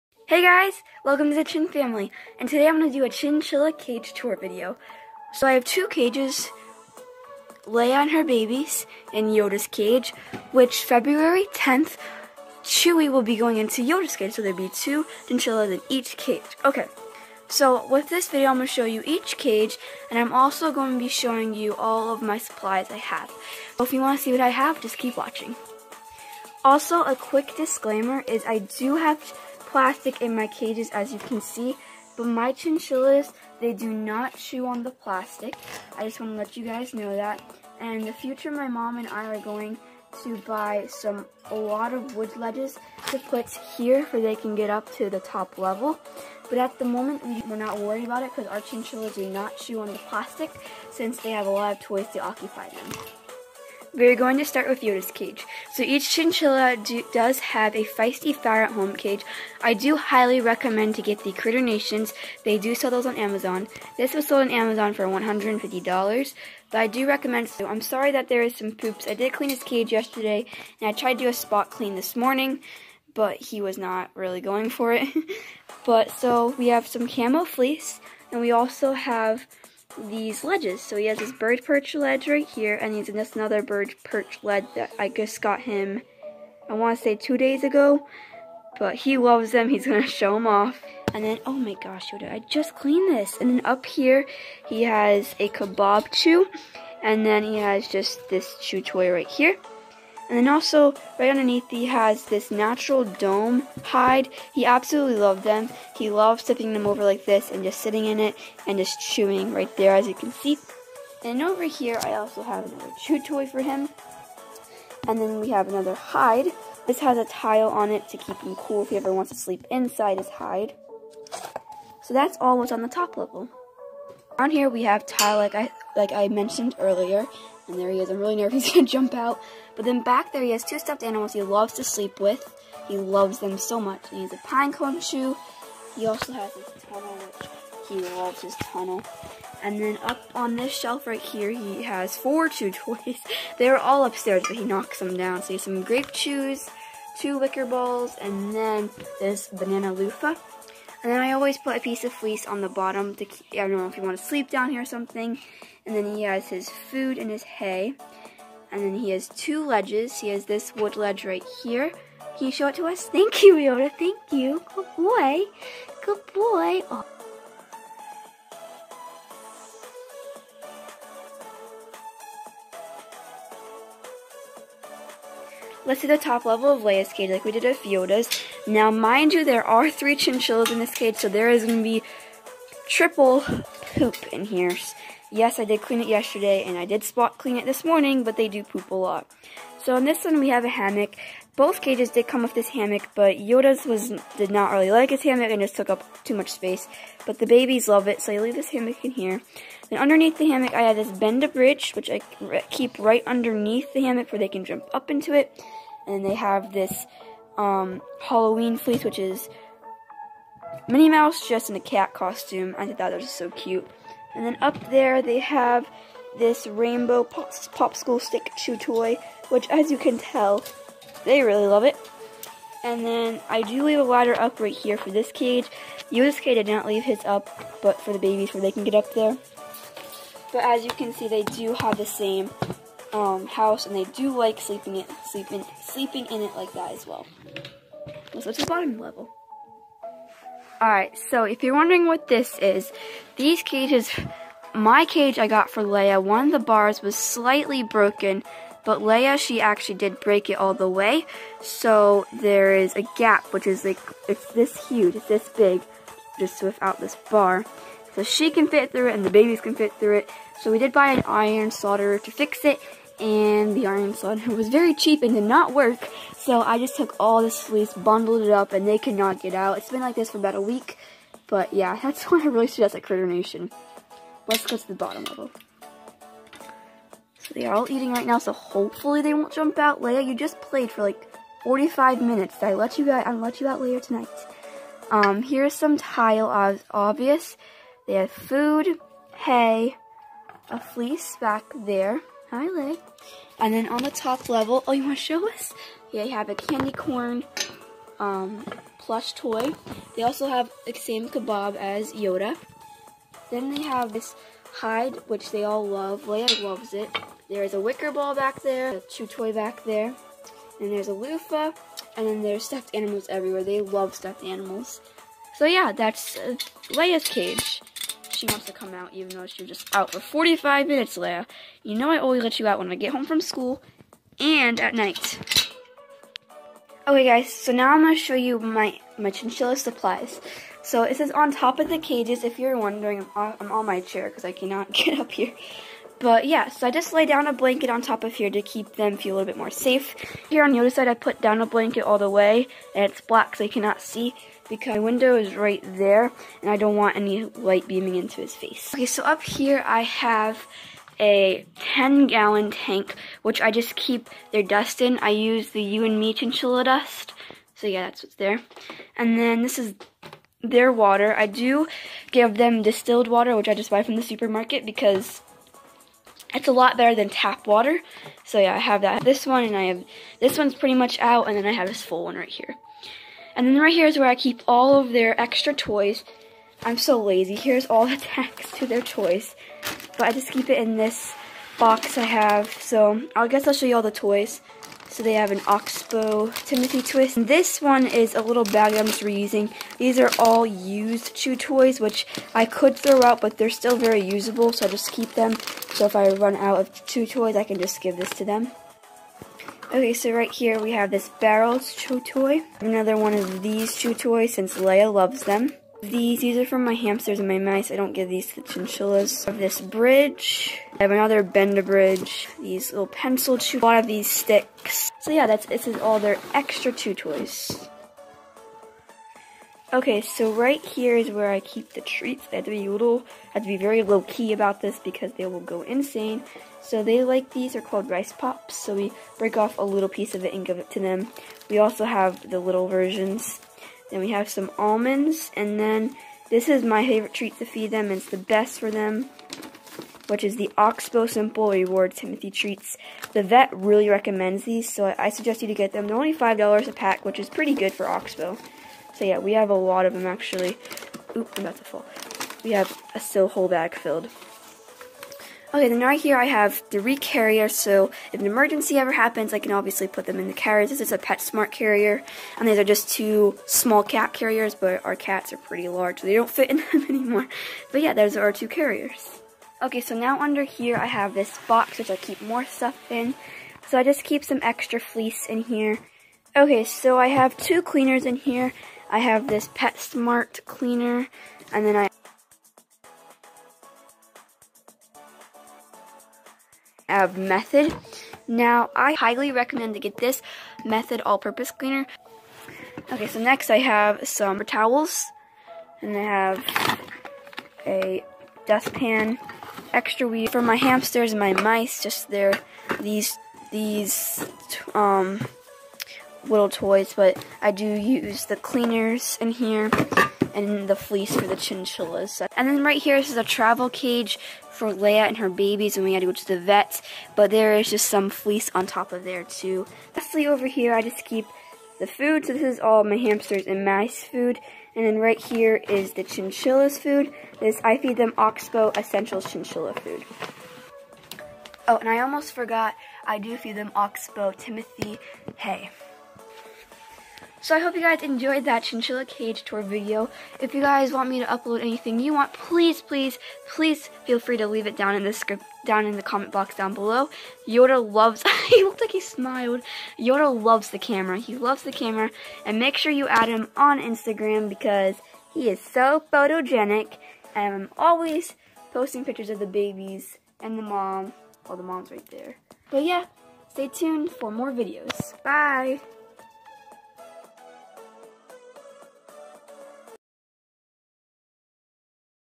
Chinchilla cage tour||2019
Hey guys, today's video I give you a tour of the chinchilla cages and all my supplies. I love chinchillas so much I have four of them Yoda, Leah, Ray, and chewy. I hope you enjoyed today's video, make sure you go check out my first video. Guess I know I have plastic in the cages the chinchillas do not chew at it though but in the future I will be buying wood ledges.

💓I have 10 pets
💓I love all animals